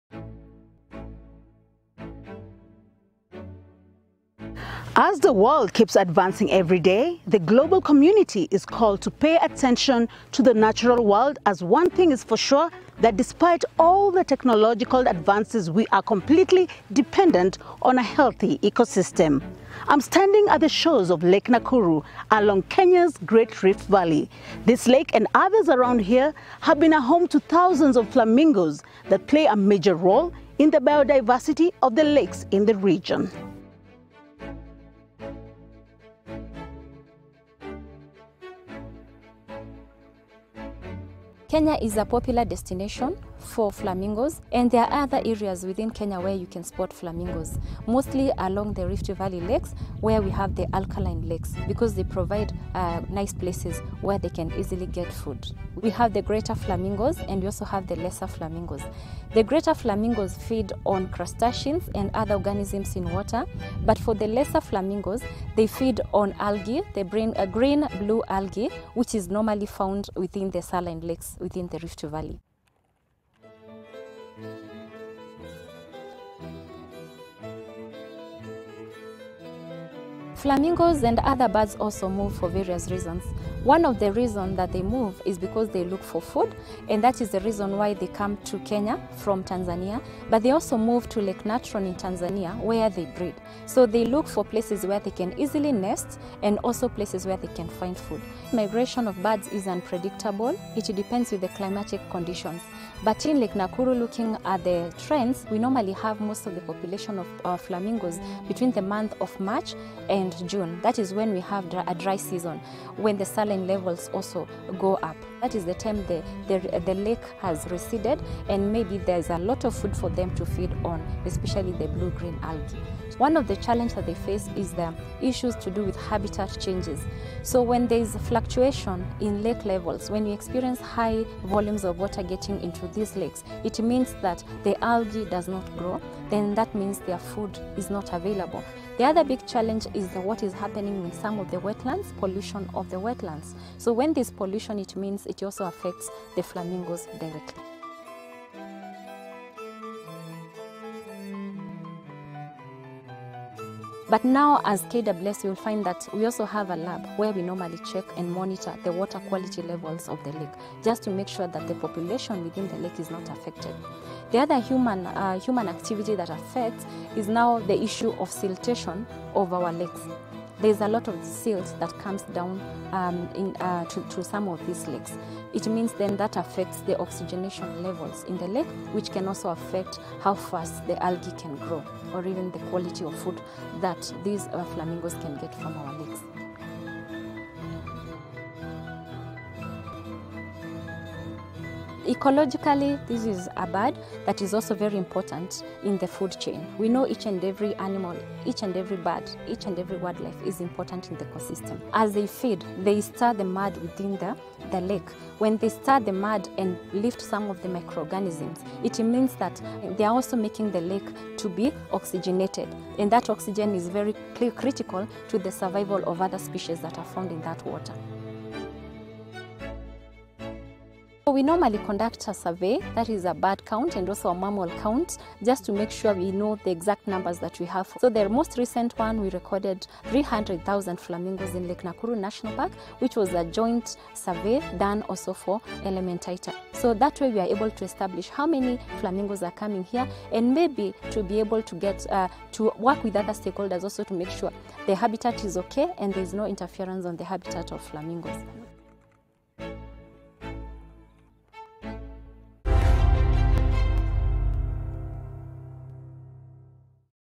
GLOBALink | Kenyan scientists sound alarm over threats to flamingos
The human and climate induced threats to flamingos that inhabit Kenya's alkaline Rift Valley lakes have mounted, hence diminishing the migratory birds' population. Efforts are being made to counter the situation.